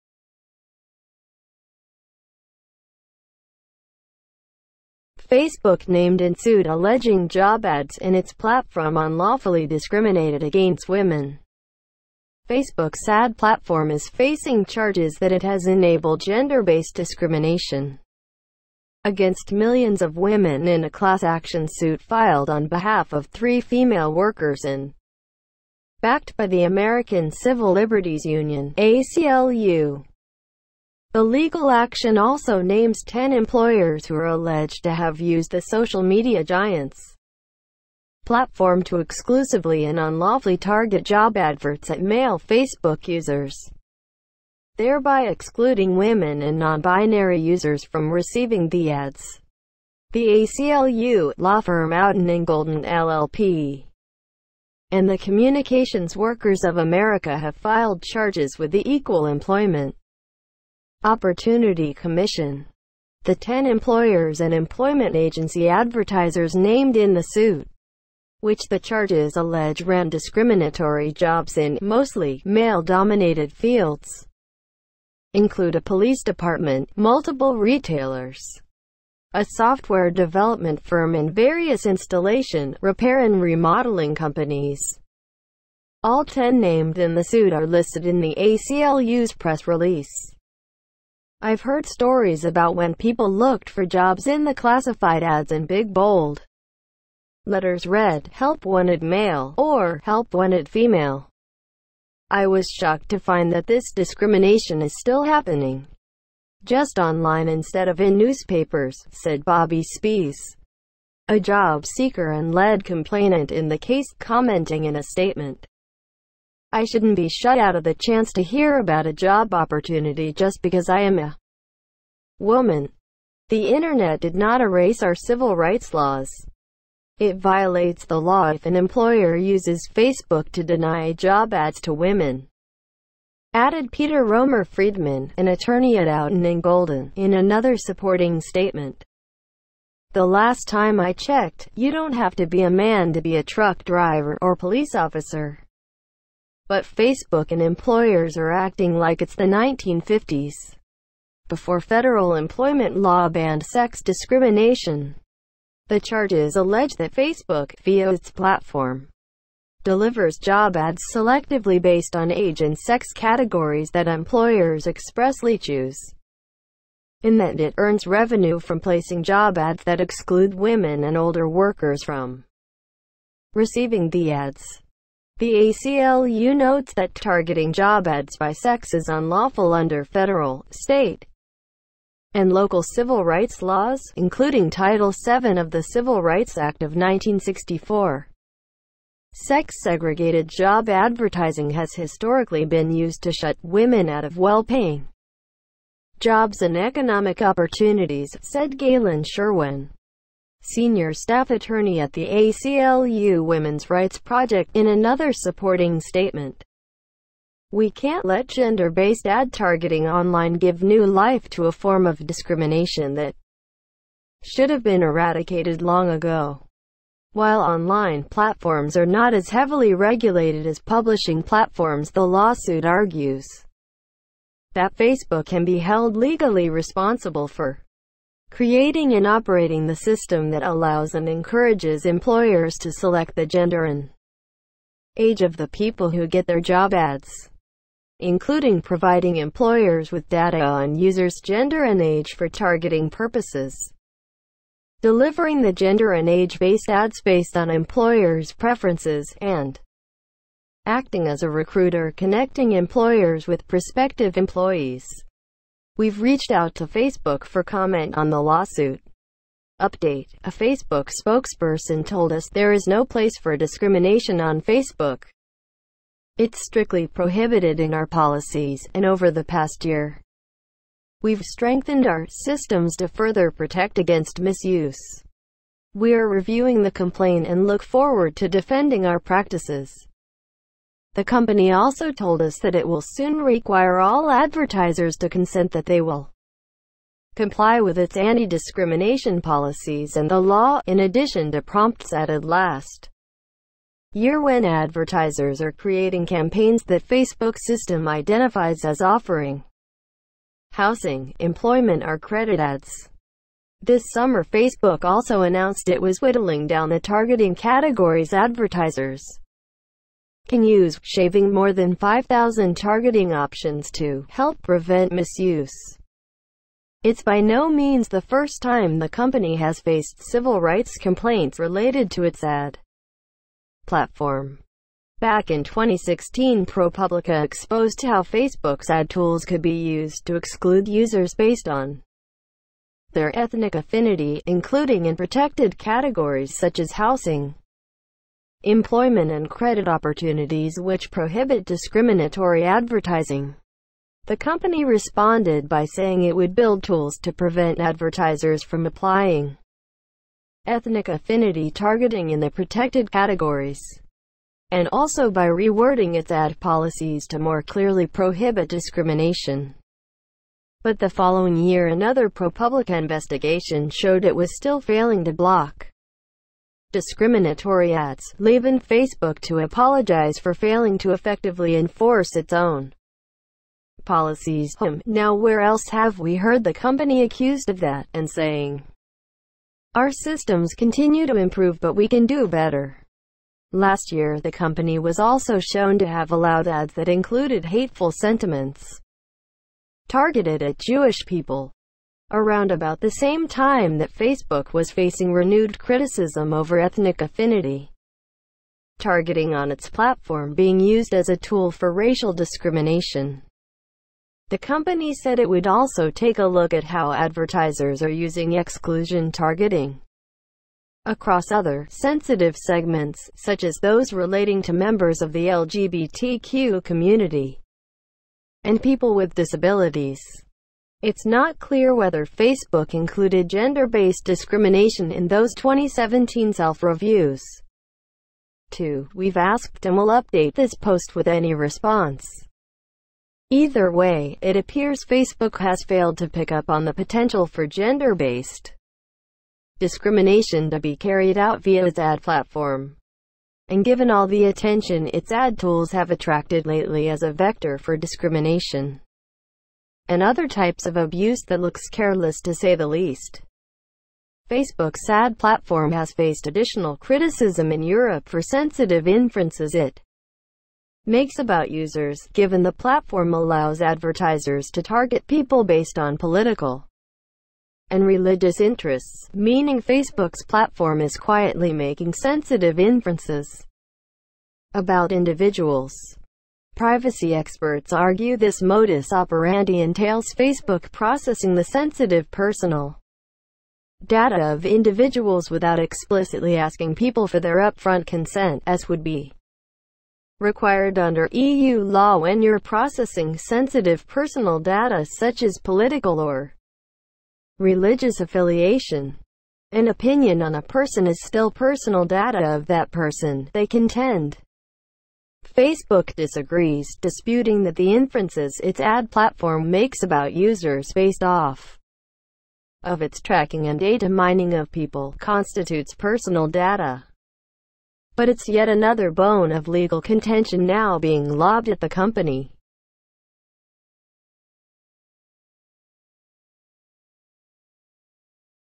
[Tech Today Bro] Facebook named alleging job unlawfully discriminated againts women.
Facebook’s  ad platform is facing charges that it has enabled gender-based discrimination against millions of women in a class action suit filed on behalf of three female workers and backed by the American Civil Liberties Union (ACLU).

The legal action also names ten employers who are alleged to have used the social media giant’s platform to exclusively and unlawfully target job adverts at male Facebook users, thereby excluding women and non-binary users from receiving the ads.

The ACLU, law firm Outten & Golden LLP, and the Communications Workers of America have filed charges with the Equal Employment Opportunity Commission.